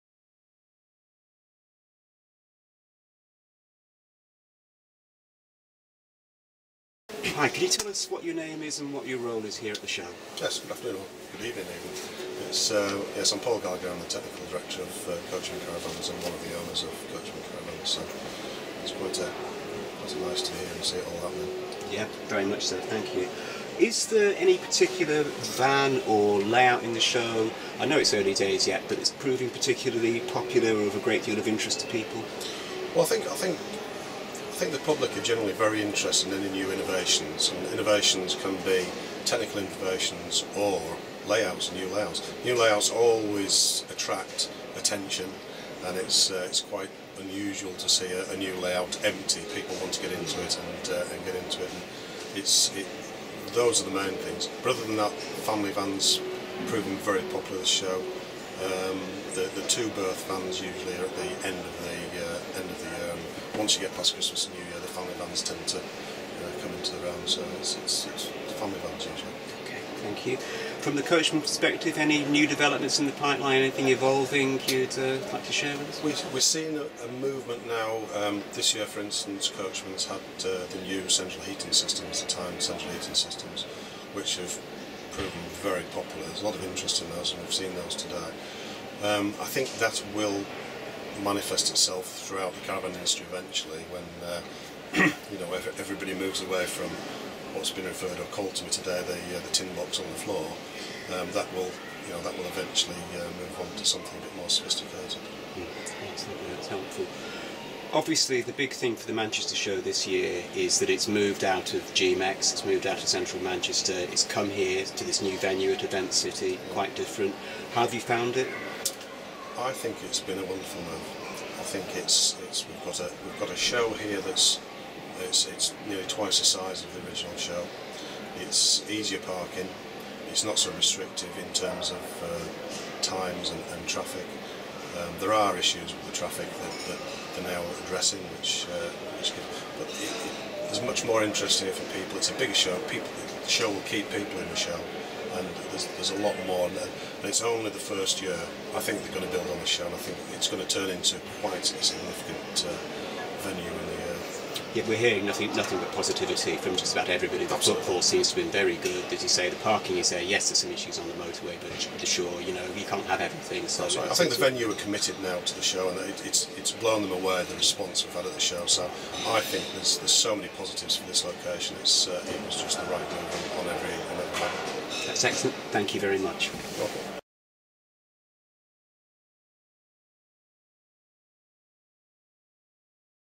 Manchester Caravan Show 2012 - Paul Gallagher of Coachman
We interview Paul from Coachman Caravans about their view of the market and the way forward.